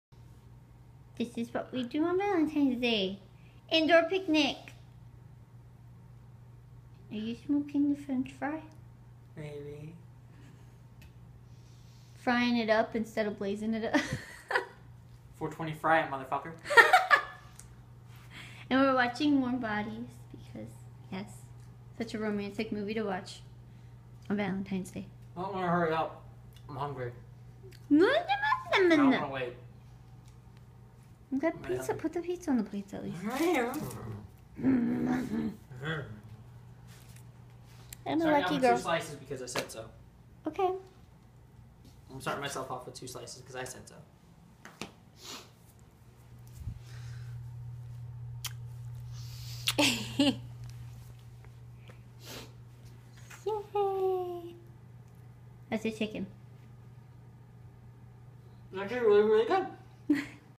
Vlog #15 Valentine's Day Indoor-Picnic Date with my Babe (Weekend at Cal U)
So we were originally gonna have our meal and get Domino's but they weren't delivering there anymore so we settled on a local pizza joint and it was so worth it. The simplest things can be so romantic.